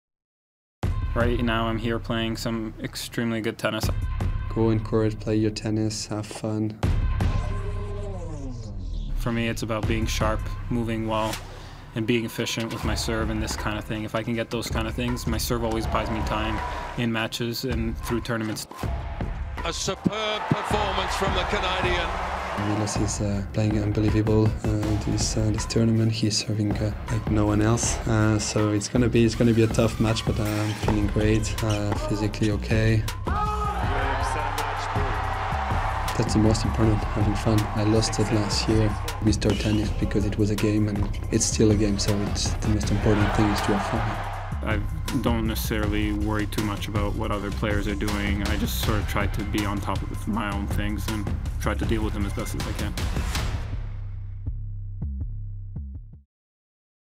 Battle Thoughts: Milos Raonic v Lucas Pouille | Australian Open 2019
A semifinal awaits for the winner of the quarterfinal match between Milos Raonic and Lucas Pouille at the 2019 Australian Open.